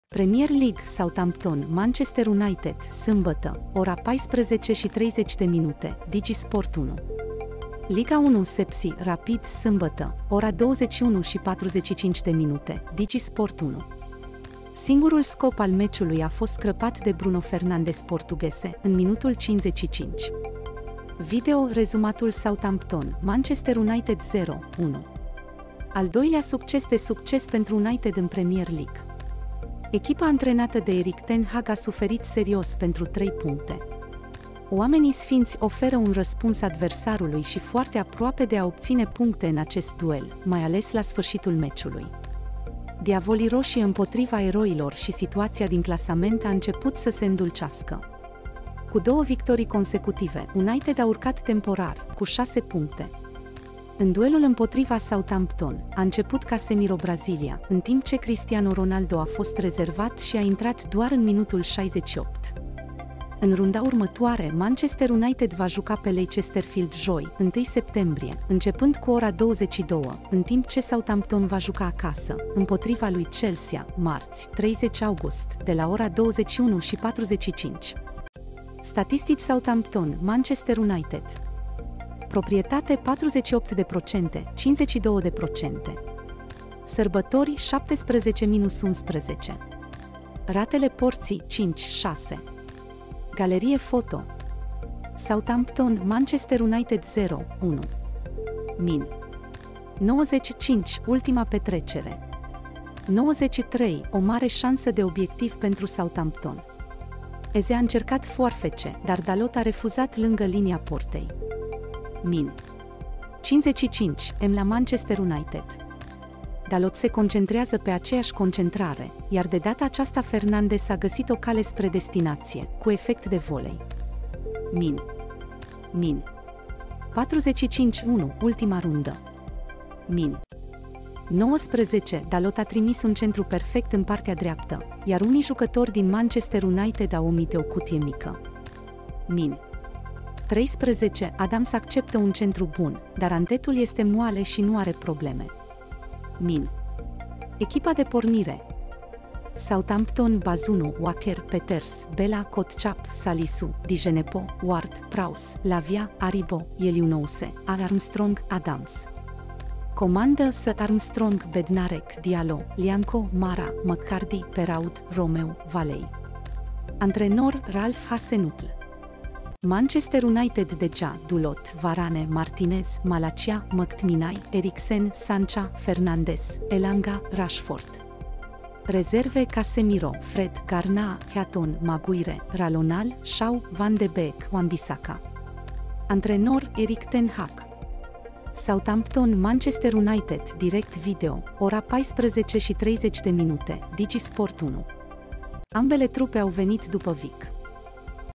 Southampton - Manchester United 0-1. Bruno Fernandes a adus trei puncte la „Red Devils”
Faza numărul 4 în debutul din Premier League sâmbătă la Digi Sport, cu vizita lui Manchester United pe terenul Southampton. Southampton - Manchester United a jucat la ora 14:00 și poate fi vizionat în videoclip direct pe Digi Sport 1, dar și pe sau în aplicația Digi Sport [...]
Premier League: Southampton - Manchester United, sâmbătă, 14:30, Digi Sport 1

Liga 1: Sepsi - Rapid, sâmbătă, 21:45, Digi Sport 1

Singurul scop al meciului a fost crăpat de Bruno Fernandes Portughese, în minutul 55. Video: Rezumatul Southampton - Manchester United 0-1

Al doilea succes de succes pentru United în Premier League

Echipa antrenată de Erik Ten Hag a suferit serios pentru trei puncte. „Oamenii sfinți” oferă un răspuns adversarului și foarte aproape de a obține puncte în acest duel, mai ales la sfârșitul meciului. „Diavolii roșii” împotriva eroilor și situația din clasament a început să se îndulcească. Cu două victorii consecutive, United a urcat temporar, cu 6 puncte. În duelul împotriva Southampton, a început Casemiro Brazilia, în timp ce Cristiano Ronaldo a fost rezervat și a intrat doar în minutul 68. În runda următoare, Manchester United va juca pe Leicester Field joi, 1 septembrie, începând cu 22:00, în timp ce Southampton va juca acasă, împotriva lui Chelsea, marți, 30 august, de la 21:00: 45

Statistici Southampton - Manchester United:

Proprietate: 48% - 52%

Sărbători: 17-11

Ratele porții: 5-6

Galerie foto

Southampton - Manchester United 0-1

Min. 90+5: Ultima petrecere! 90+3: O mare șansă de obiectiv pentru Southampton! Eze a încercat foarfece, dar Dalot a refuzat lângă linia Portei. Min. 55: Emla Manchester United! Dalot se concentrează pe aceeași concentrare, iar de data aceasta Fernandes a găsit o cale spre destinație, cu efect de volei. Min. Min. 45+1: Ultima rundă. Min. 19: Dalot a trimis un centru perfect în partea dreaptă, iar unii jucători din Manchester United au omite o cutie mică. Min. 13: Adams acceptă un centru bun, dar antetul este moale și nu are probleme. Min. Echipa de pornire:

Southampton: Bazunu - Walker -Peters, Bella -Kotchap, Salisu, Djenepo - Ward -prowse, Lavia - Aribo, Elyounouse, A. Armstrong - Adams

Comandă: S. Armstrong, Bednarek, Diallo, Lyanco, Mara, McCarthy, Perraud, Romeu, Valery

Antrenor: Ralph Hasenhuttl

Manchester United: De Gea - Dulot, Varane, Martinez, Malacia - McTminay, Eriksen - Sancho, Fernandes, Elanga - Rashford

Rezerve: Casemiro, Fred, Garnacho, Heaton, Maguire, Ralonal, Shaw, Van de Beek, Wan-Bissaka

Antrenor: Erik Ten Hag

Southampton - Manchester United, Direct Video, 14:30, Digi Sport 1. Ambele trupe au venit după Vic